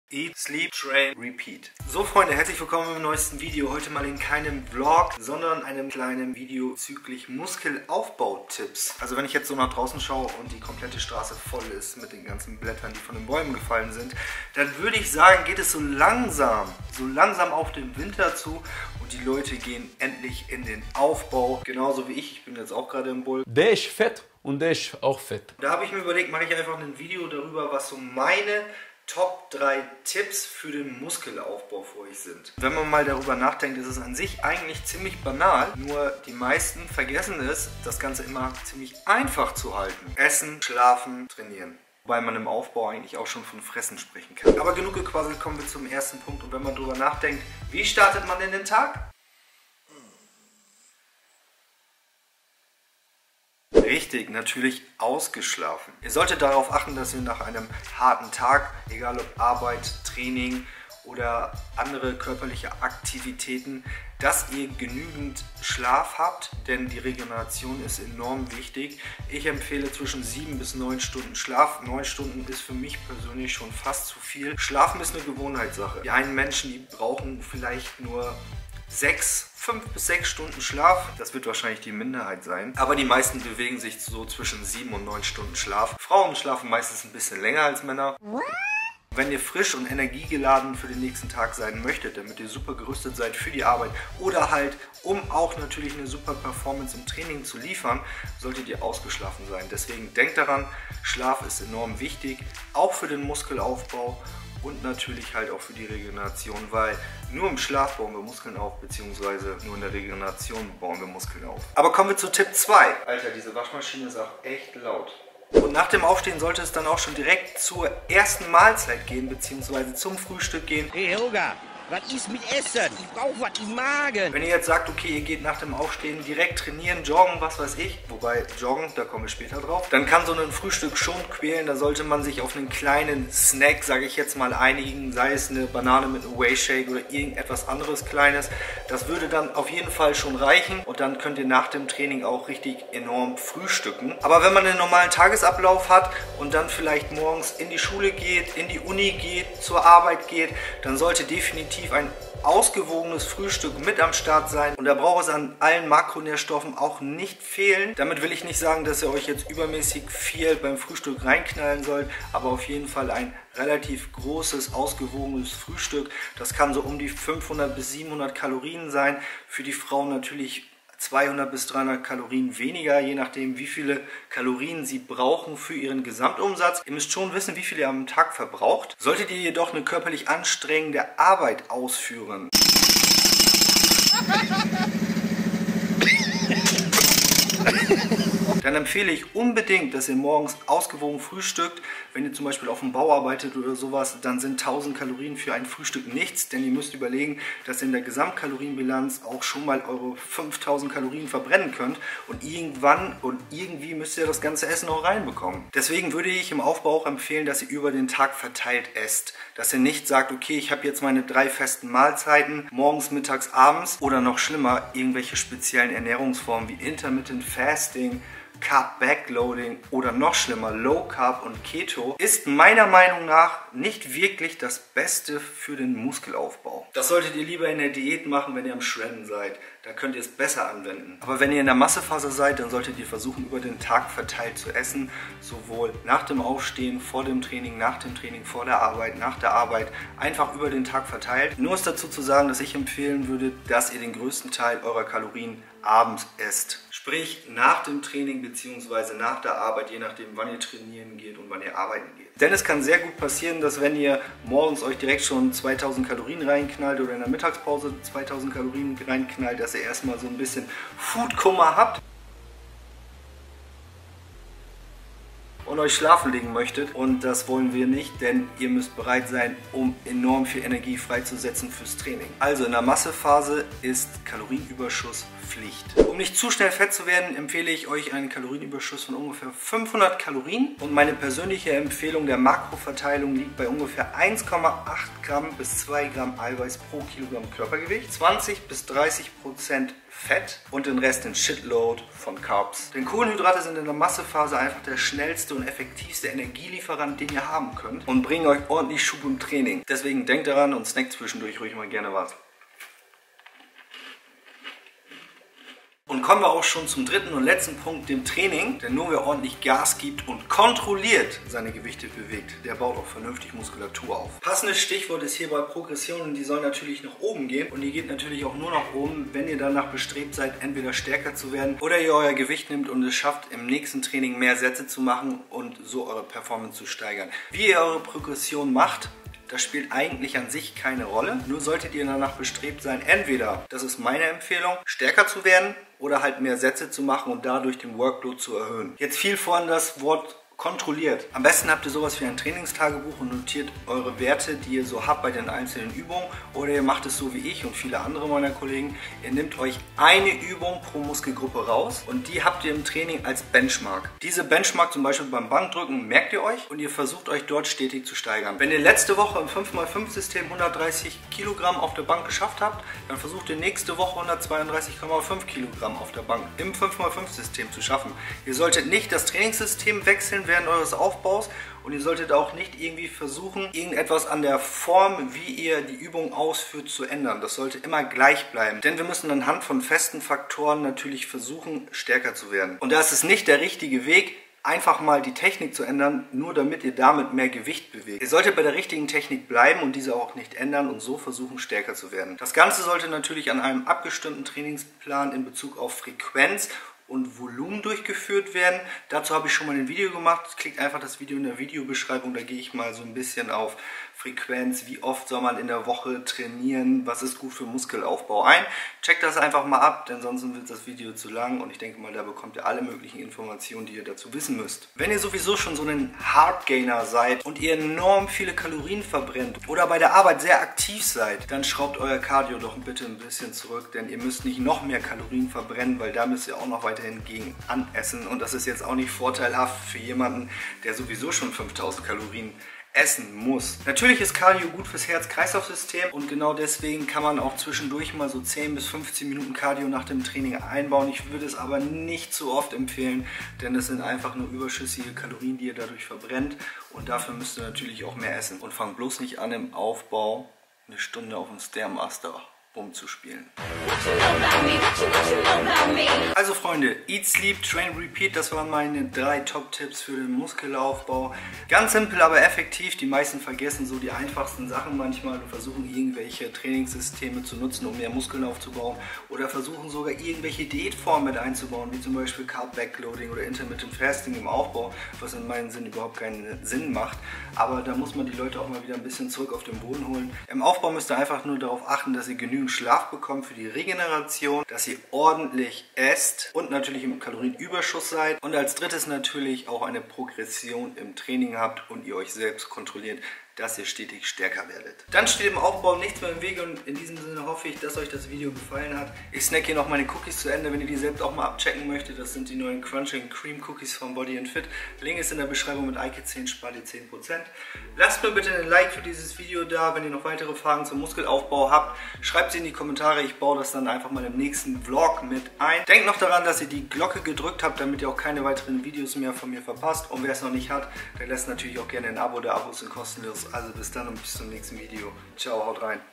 MUSKELAUFBAU - 3 wichtige Tipps | MASSE aufbauen!
Hier zusammengefasst meine 3 absolut wichtigsten Tipps für einen erfolgreichen Muskelaufbau!